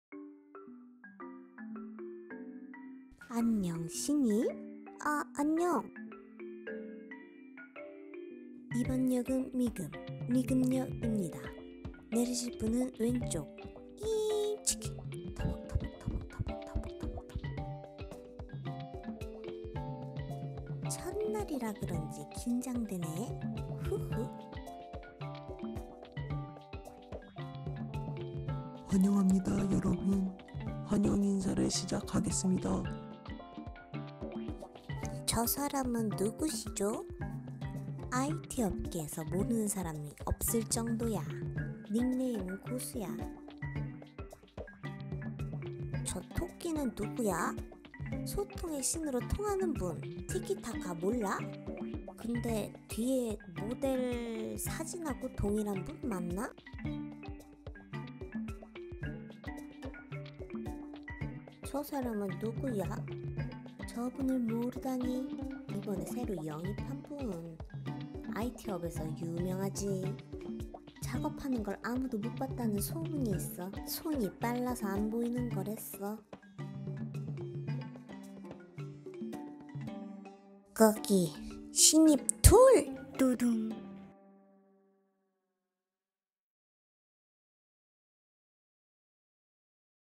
[자유롭게 티맥스 PPL 만화 : ep 3 ] 첫 출근 지린 썰, 티키타카 첫 대면 그리고 TeeSpace?
📼[자유롭게, 티맥스 PPL 만화 ] 에피소드 3화:
(티키타카,티스페이스,TeeSpace, Tmax)

000회사를 합격하게 된 구르미와 아무개,
둘은 신입 환영식을 가기 위해 지하철을 탄다.
신입인지라 긴장되는 두근거림을 안고 환영식에 도착하고.
그곳에서 IT업계 대선배와 마주하게 되는데..